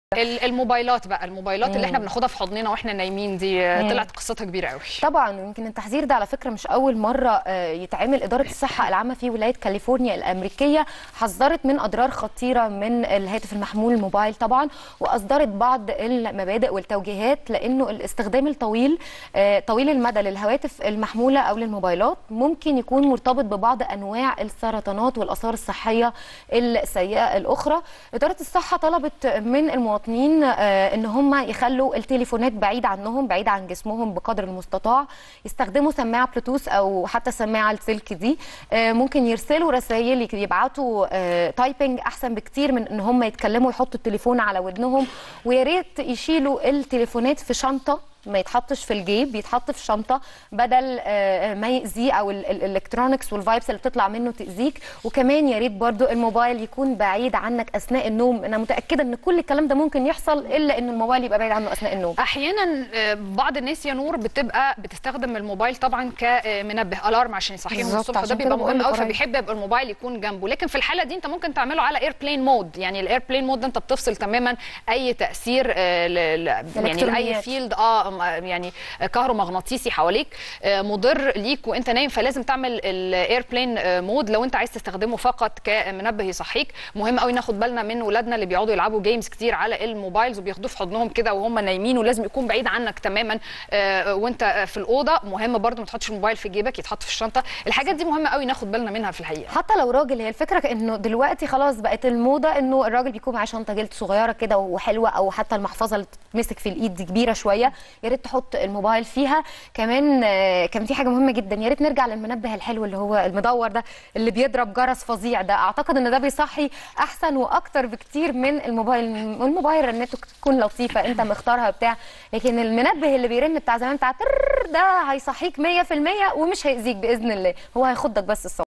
الصحة العامة في كاليفورنيا تحذر مواطنيها.. إشعاعات التليفون المحمول قد تسبب السرطان
أصدرت كاليفورنيا إرشادات للحد من التعرض لإشعاع الموبايل، وسط أدلة متزايدة على أن استخدامه قد يكون مرتبط بالسرطان، ويسبب ضعف الانتباه، ومشاكل الصحة النفسية والصحة الإنجابية "العقم".
 وأوضح التقرير أن الهواتف المحمولة تقوم بنقل المعلومات باستخدام إشارات لاسلكية منخفضة التردد، ما قد يعرضنا للإشعاع غير الصحى، خاصة عند إرسال الملفات الكبيرة أو تحميلها، وفقاً لوزارة الصحة الأمريكية.